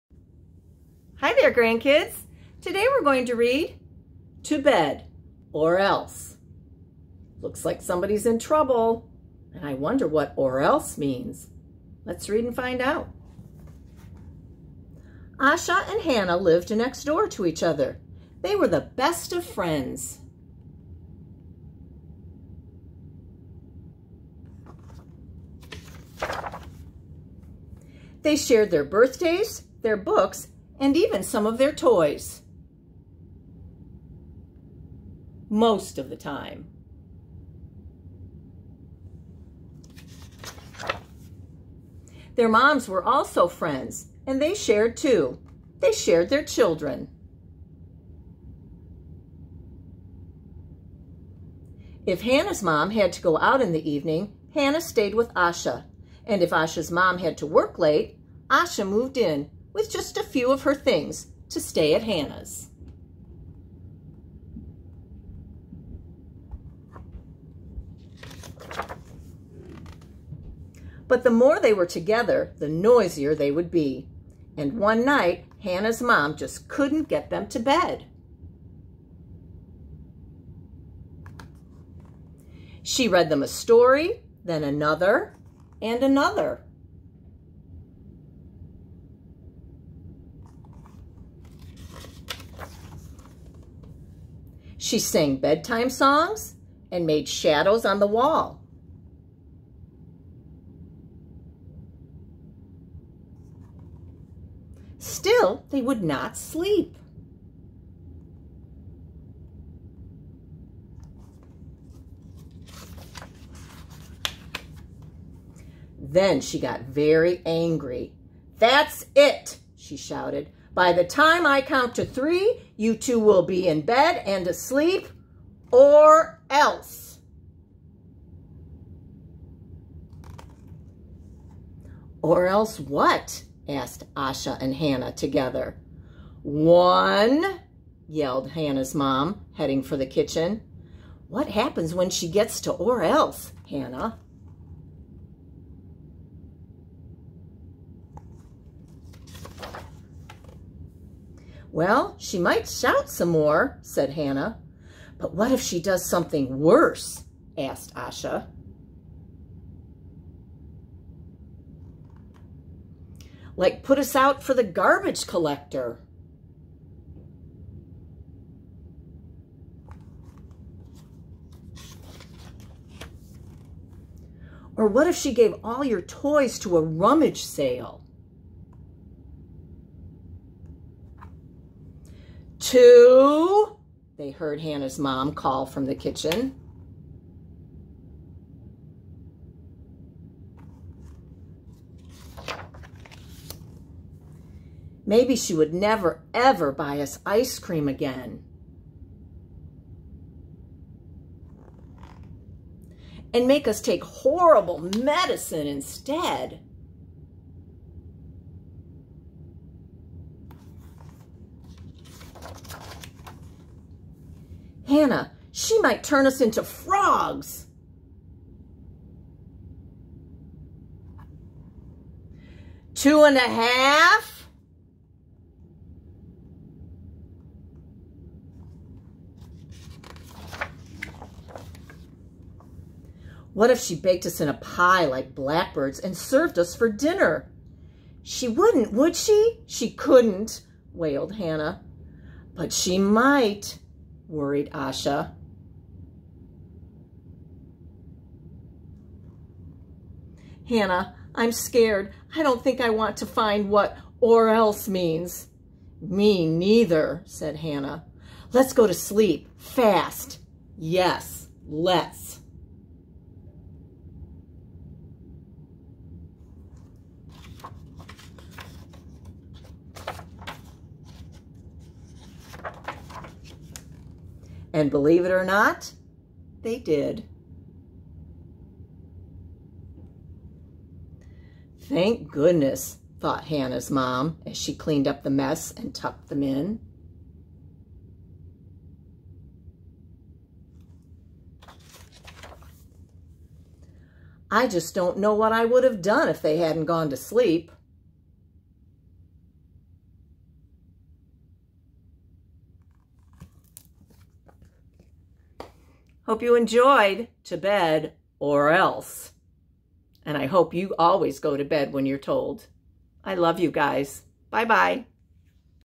To bed... OR ELSE!
Asha and Hannah just won't go to sleep!